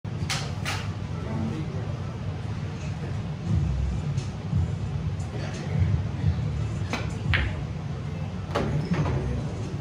670 By The Way Bank
Had to push the cue ball past the bank angle, hence "by the way".